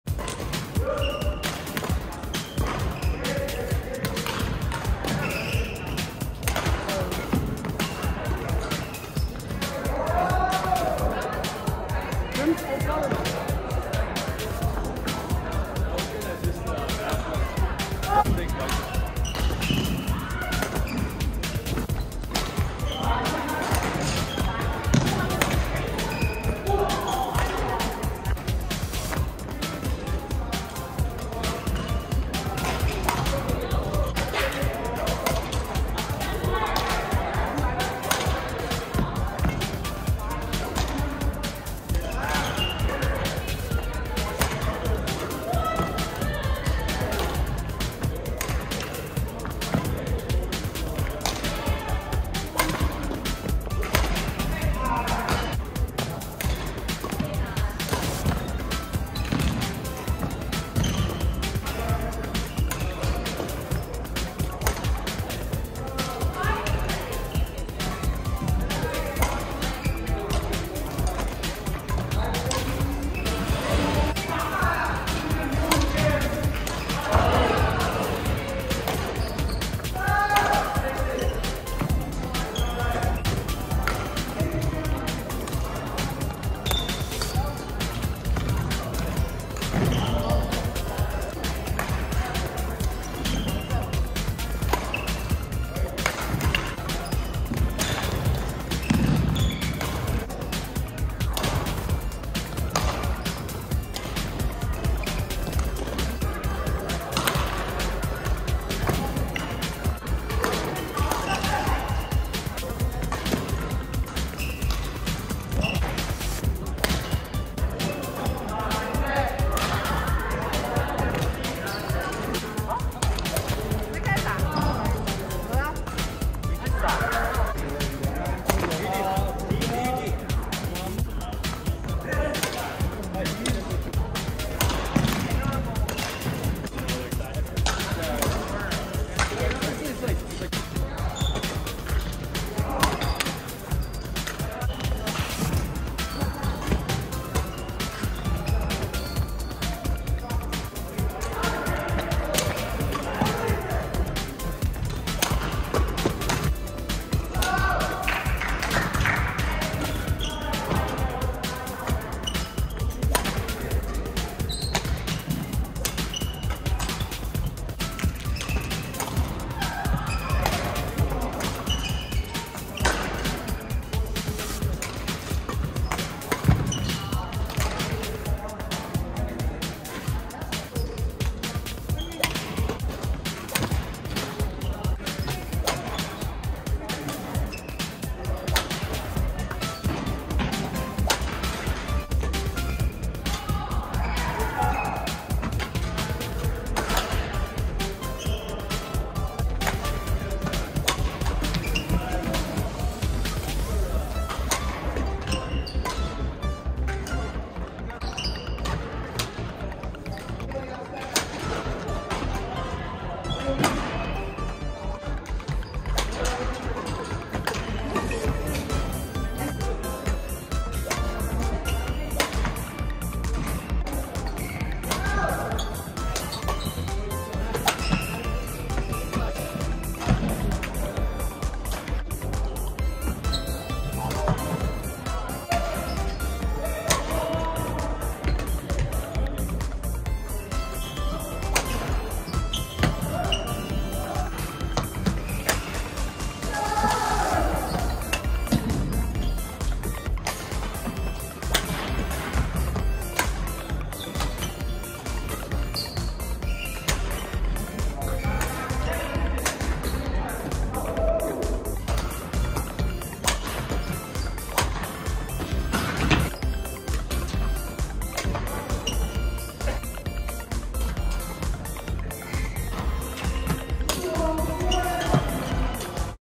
Badminton Shots Year 3 Week 16 - Better form !
Looking to improve my form by using the left arm properly. I normally lose balance because my left arm flops to the side and also I need to use the left arm to pull the right side forward
I focused my on drops and  softer shots coupled with better recovery footwork and rotation
I felt like I saw a lot of improvement in just one day